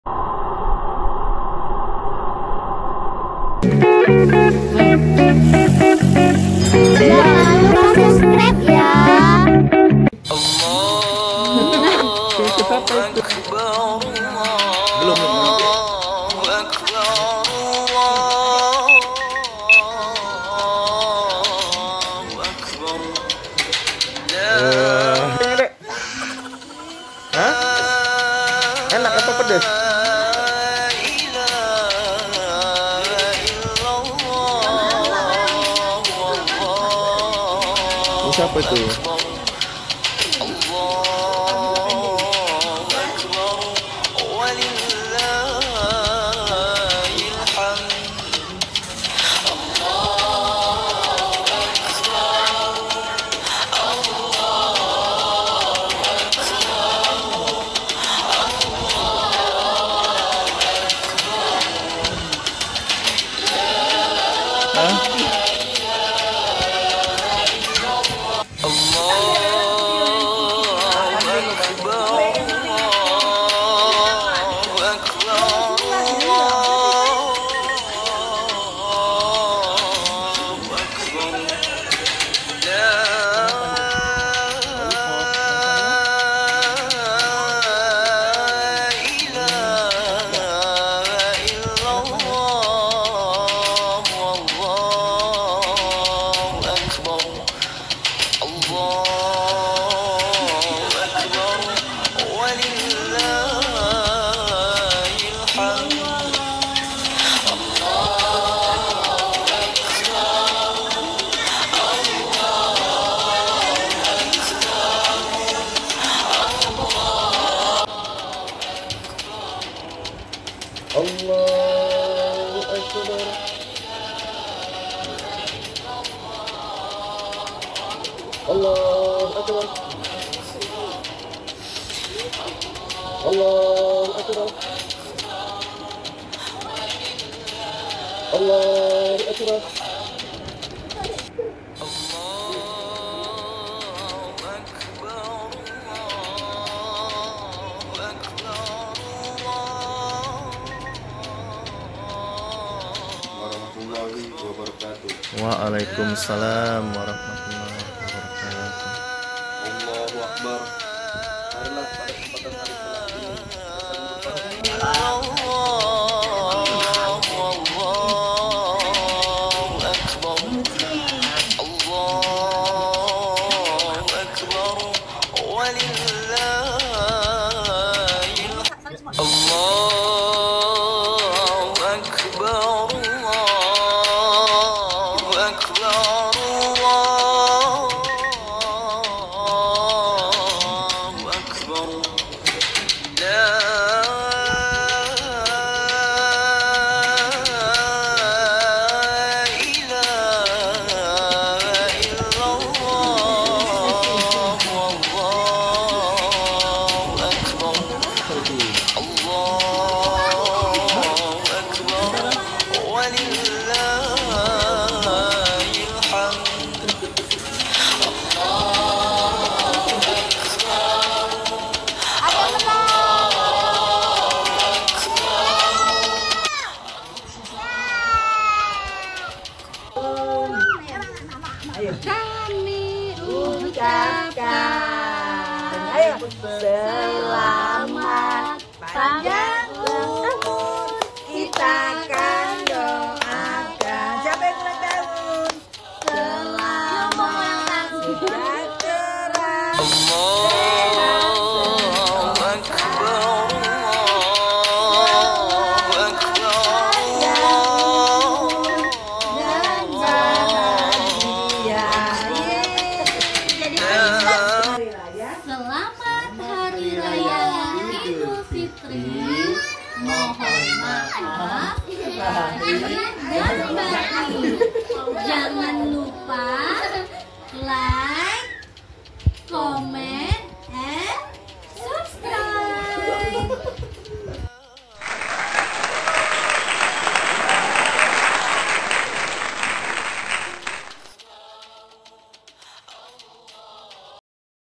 INDAHNYA LEBARAN IDUL FITRI 1441 H | MOHON MAAF LAHIR BATIN
Idul fitri 1441 H tetap terasa syahdu meskipun sedang dalam kondisi pandemic covid-19..

Mohon maaf lahir batin.. selamat lebaran idul fitri..